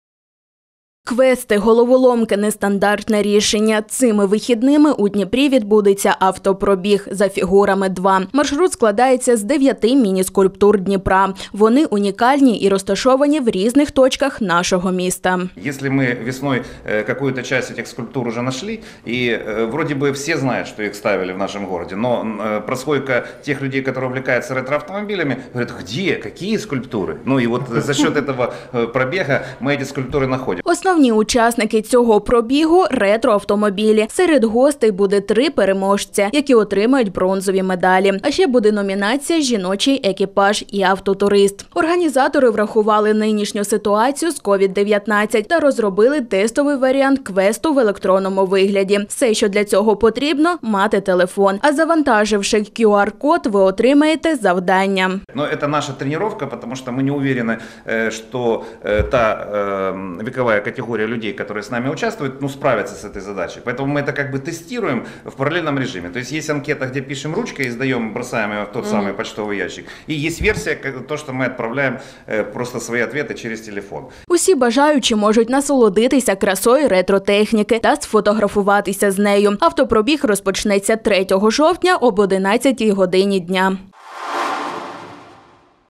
У Дніпрі відбудеться осінній автопробіг “За фігурами-2”
Квести, головоломки, нестандартне рішення. Цими вихідними у Дніпрі відбудеться автопробіг “За фігурами -2”. Маршрут складається з 9 мініскульптурок Дніпра. Вони унікальні і розташовані в різних точках нашого міста.